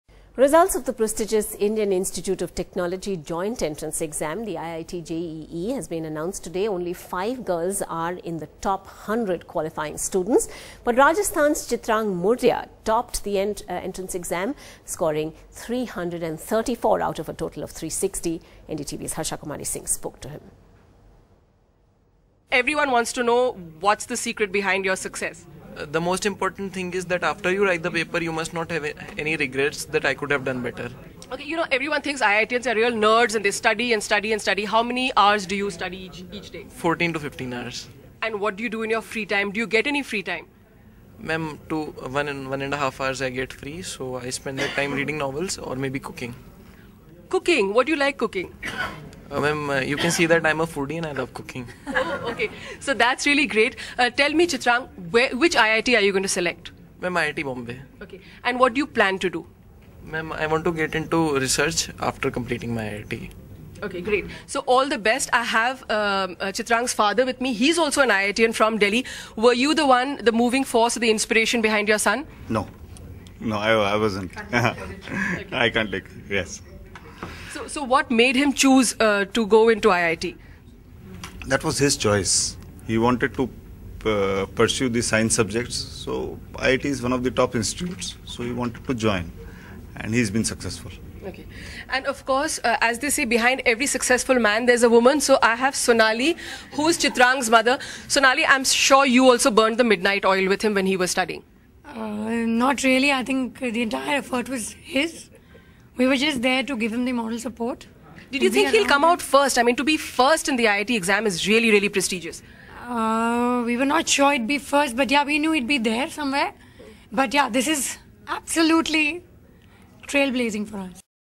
How smart is this 17-year-old? He topped IIT entrance exam
A 17-year-old from Udaipur in Rajasthan has topped the IIT entrance exam. How smart is Chitraang Murdia?  He scored 334 marks out of 360.